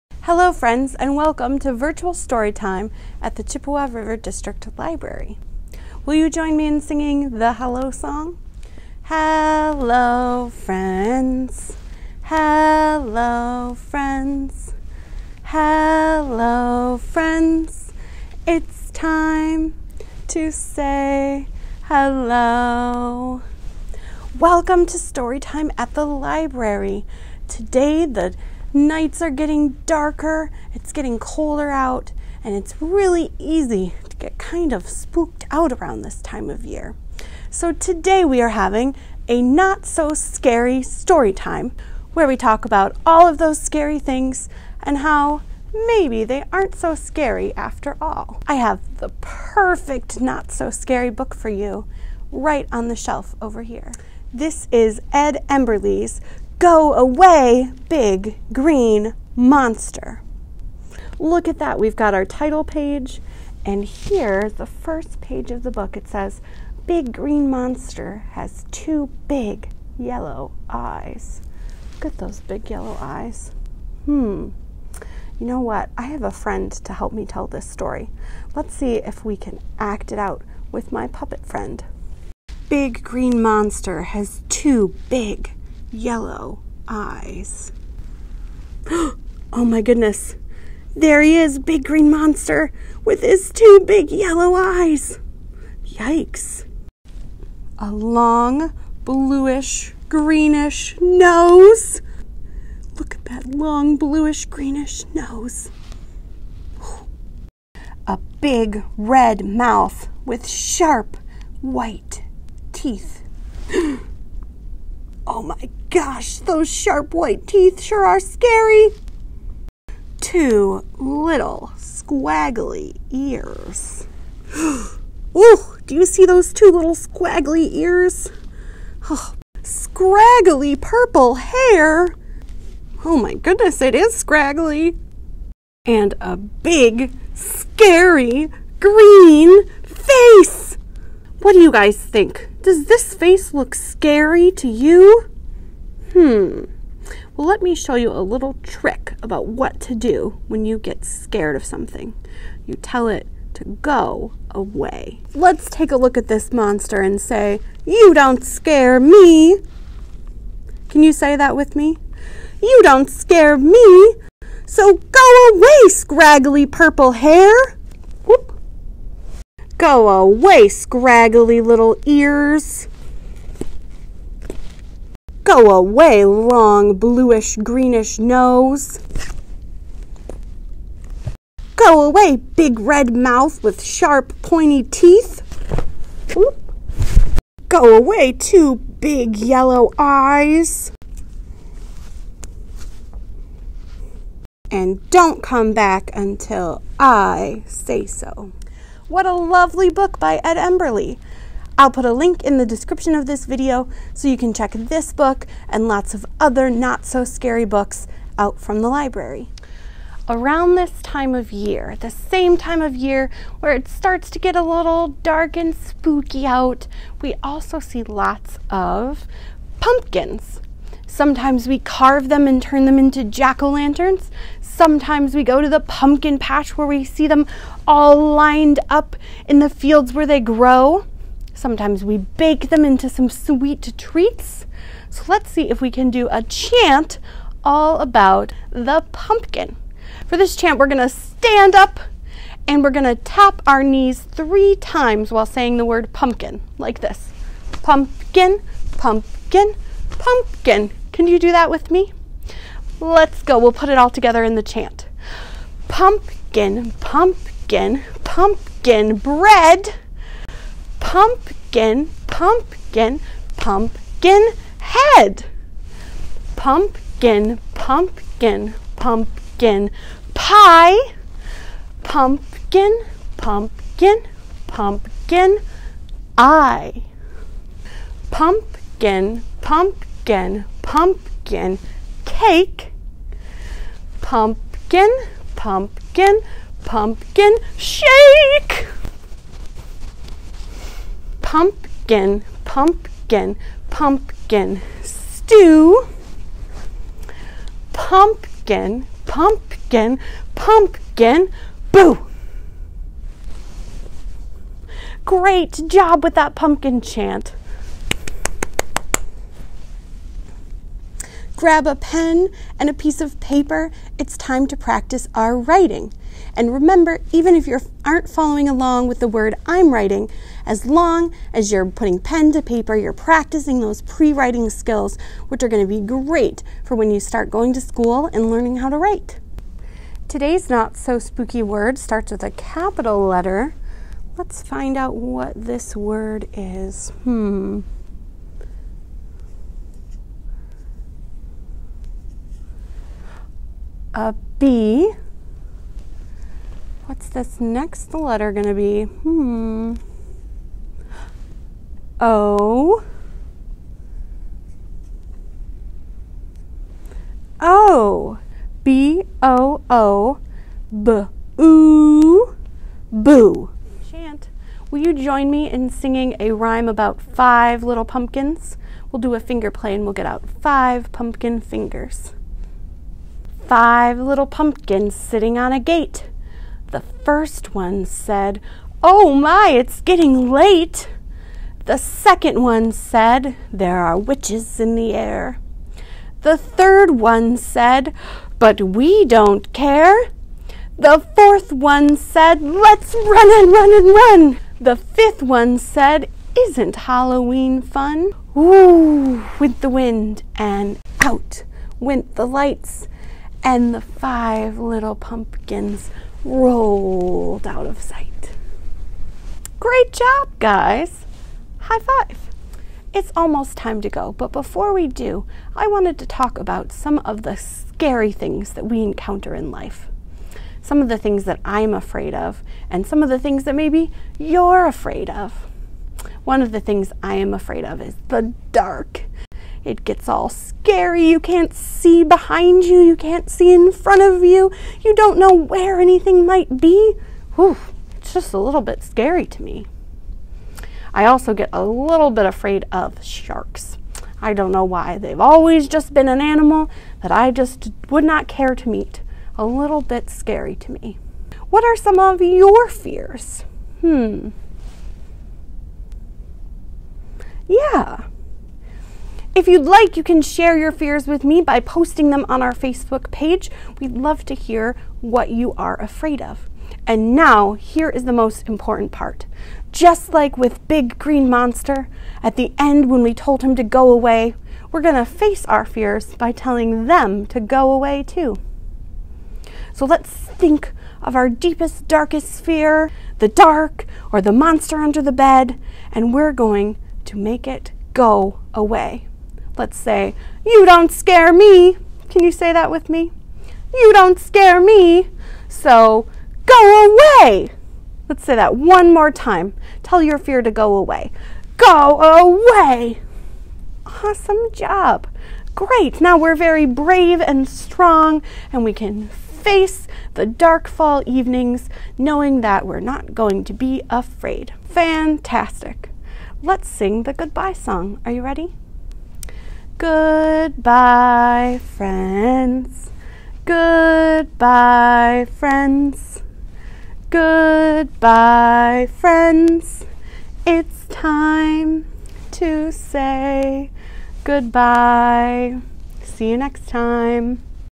Not So Scary Storytime
There's no need to fear, a Not So Scary Storytime is here! Come read, write, talk, play and sing with us at the Chippewa River District Library  in our latest spooky themed storytime!